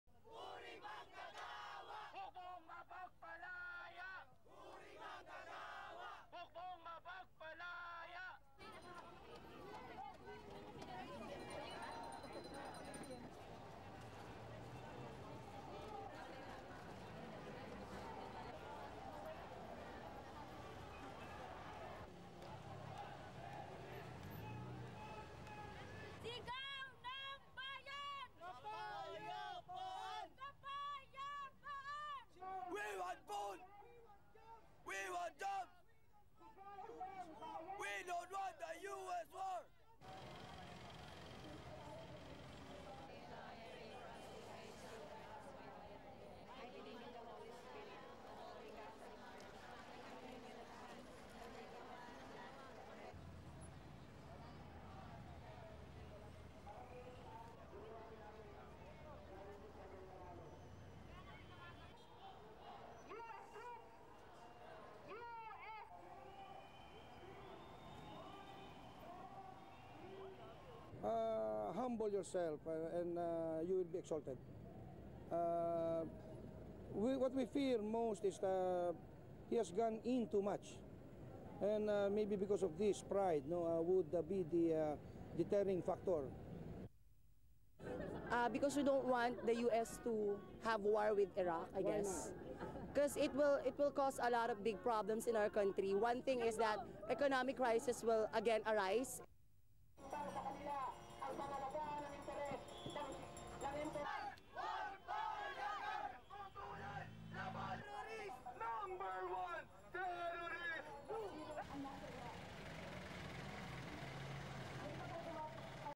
Antiwar rally organised by Catholic church in Manila
1. Huge banner "No to war, No to US Aggression", leading march
2. Protesters holding placard that reads: "Filipinos against war"
3. Wide shot of anti-war marchers, including nuns
4. Reverse shot of anti-war march
5. Wide shot protesters
6. Protesters marching holding banner "Condemn US war of aggression in Iraq"
7. Male protester with peace sign painted on  face
8. Priests among protesters
9. Nuns praying during protest
10. Protesters, then pan to placard "No to Bush, Yes to peace"
11. Wide shot protest
12. Statue of Virgin Mary mounted on a jeep
13. SOUNDBITE: (English) Pastor Saycon, Civic Leader:
"(Message to US President George W. Bush) Humble yourself and you will be exalted.  What we feel most is that he (Bush) has gone in too much, and maybe because of this, pride will be the determining factor."
14. SOUNDBITE: (English) Serafin Sabala, Protester:
"We don't want the US to have war with Iraq, because it will cause a lot of big problems in our country.  One thing is that an economic crisis will again arise."
15. Banner "Oppose US Terrorism, Reject GMA (President Gloria Macapagal Arroyo's) support to the US war on Iraq"
16. Various protesters holding banners
17. Police at protest
18. Protester wearing headband "US imperialist number 1 terrorist"

STORYLINE:

Thousands of religious and civic leaders gathered for an anti-war march in Manila on Friday, denouncing military action against Iraq and the policies of US President George W. Bush.

Priests, nuns and students attended the protest, which was organised by the Catholic Church.

Demonstrators chanted against possible US-led military action in Iraq, and voiced concern that many innocent lives would be lost if there was an attack.

As they walked towards a bayside park near the US Embassy, the protesters prayed for peace.

The protest was also critical of Philippine President Gloria Macapagal Arroyo - with many demonstrators saying she should focus on the country's economy rather than on military action against Iraq.